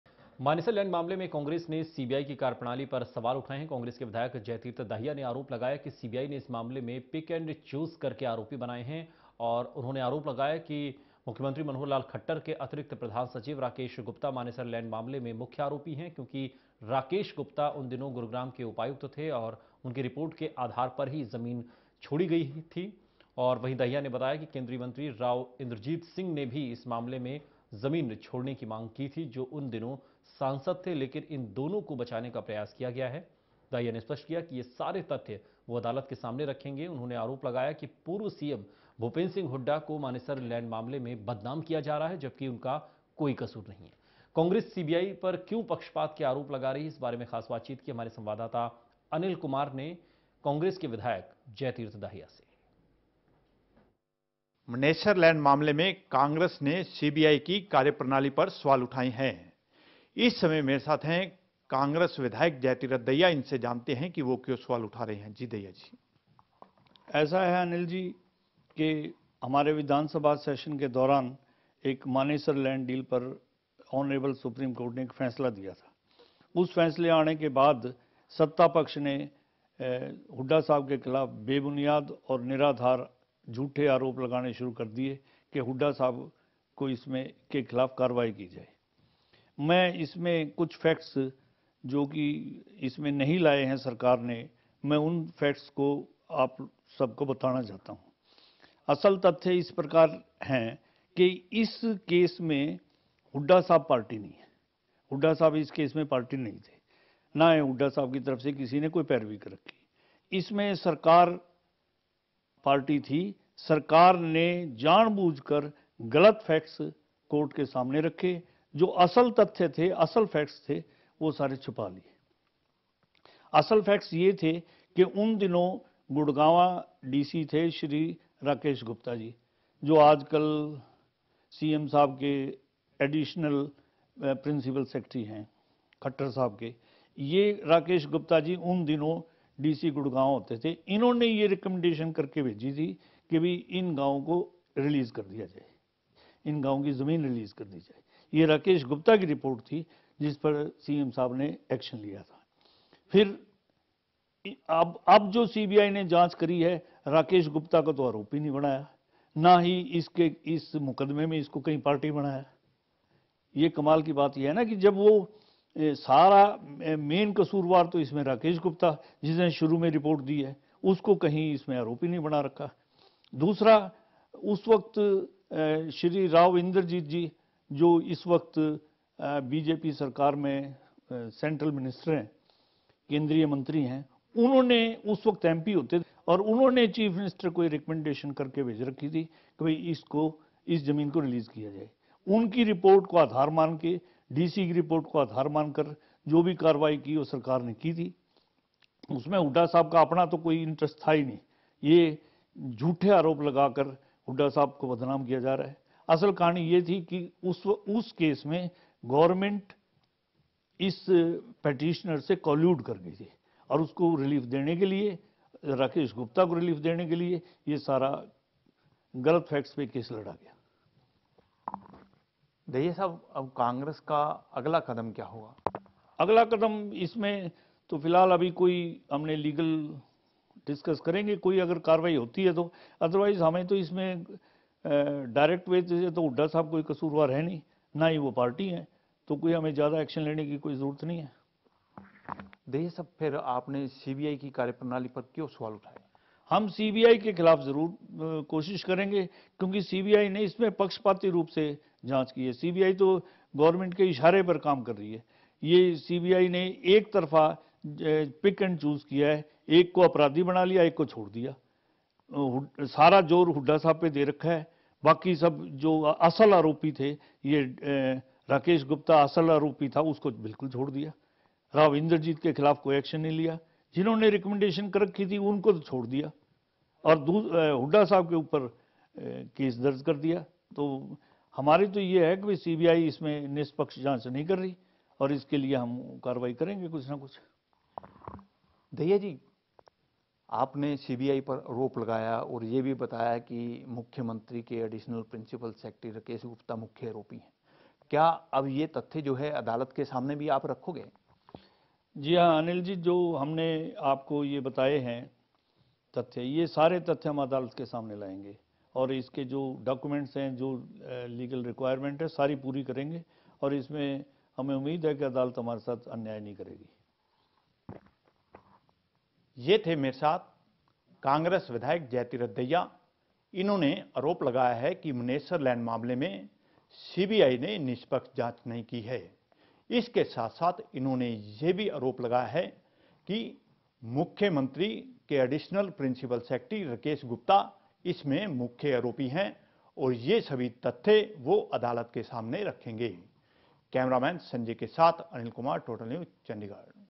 Manesar Land Deal मामले में Hooda पर लगे आरोपों पर क्या बोली Congress ?
मानेसर लैंड मामले में कांग्रेस ने सी बी आई की कार्यप्रणाली पर सवाल उठाये है. कांग्रेस विधायक जय तीर्थ दहिया ने आरोप लगाया है कि सीबीआई ने इस मामले में पिक एन्ड चूज़ करके आरोपी बनाये गए है. उन्होंने आरोप लगाया है कि मुख्यमंत्री मनोहर लाल खट्टर के अतिरिक्त प्रधान सचिव राकेश गुप्ता मानेसर लैंड मामले में मुख्य आरोपी है क्योकि राकेश गुप्ता उन दिनों गुरुग्राम के उपायुक्त थे और उनकी रिपोर्ट के आधार पर ही जमीन छोड़ी गई थी. वही दहिया ने बताया कि केंद्रीय मंत्री राव इंदरजीत सिंह ने भी इस मामले में जमीन छोड़ने की मांग की थी जो उन दिनों सांसद थे लेकिन इन दोनों को बचाने का प्रयास किया गया है. दहिया ने स्पष्ट किया कि ये सारे तथ्य वो अदालत के सामने रखेंगे। उन्होंने आरोप लगाया कि पूर्व मुख्यमंत्री भूपिंदर सिंह हुड्डा को मानेसर लैंड मामले में बदनाम किया जा रहा है जबकि उनका कोई कसूर नहीं है. कांग्रेस सीबीआई पर क्यों पक्षपात के आरोप लगा रही है. इस बारे में खास बातचीत की हमारे सवांददाता अनिल कुमार ने कांग्रेस विधायक जय तीर्थ दहिया से.